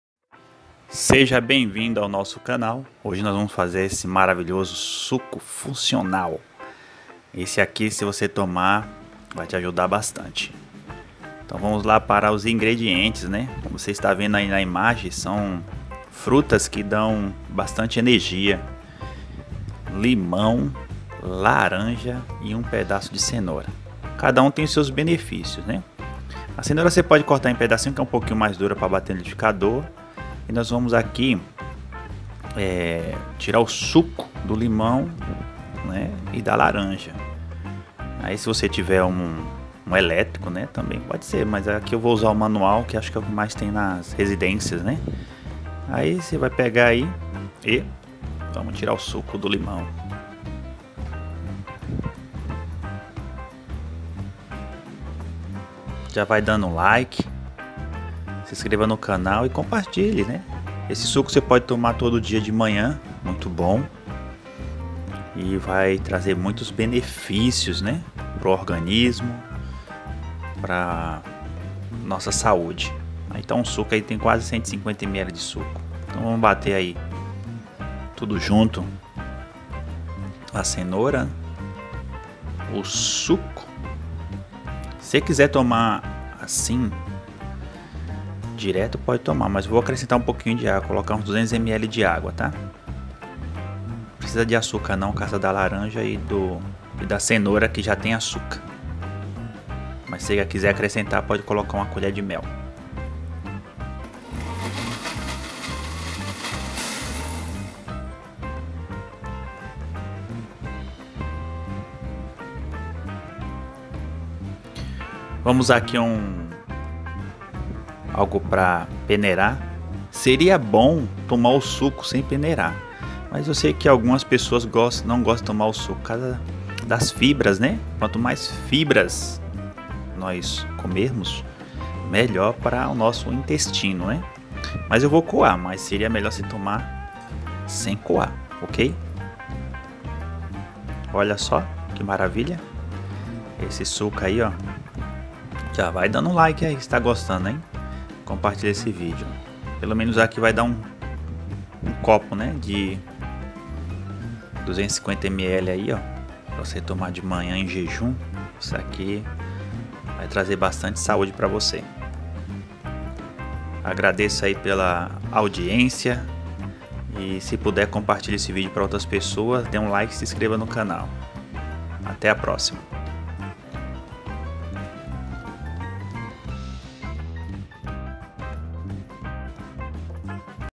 SUCO FUNCIONAL: LARANJA CENOURA E LIMÃO
Quais os benefícios do suco de laranja com cenoura e limão?

A nutricionista destaca como o suco feito a partir desses tres ingredientes pode auxiliar na limpeza do organismo: "Ajuda no trabalho de detoxificação feito pelo fígado, acelera o metabolismo, ajuda a pele a respirar e contribui para o funcionamento do intestino"

Qual o melhor horário para tomar suco de cenoura com laranja?
- Tomar o suco pela manhã energiza o corpo e promove o bom humor. – Consumido em jejum, o suco de laranja puro purifica o corpo, elimina os resíduos e melhora o ciclo intestinal. A ingestão diária reduz em pelo menos 14% as possibilidades de obesidade.

Bom Apetite

Curso da Bíblia on-line - Este curso da Bíblia é interativo, fácil de usar, sem compromisso e grátis.